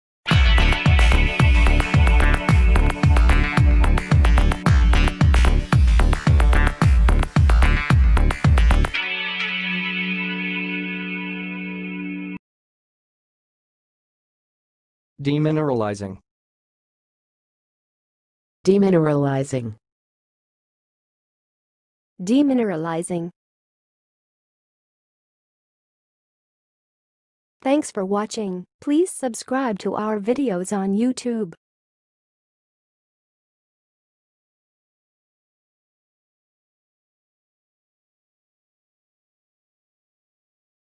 How to say "demineralizing"! (High Quality Voices)
Watch in this video how to say and pronounce "demineralizing"!